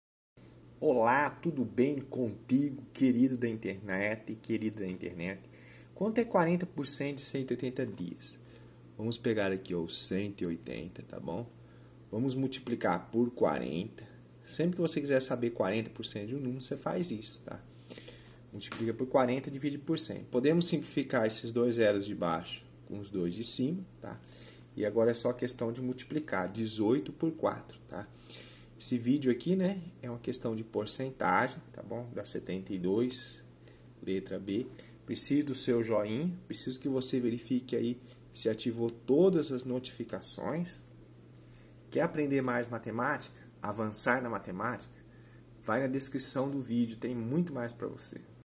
1 QUESTÃO DE PORCENTAGEM - Qual o valor de 40% de 180 dias?
Qual o valor de 40% de 180 dias?
a) 65
b) 72
c) 80
d) 120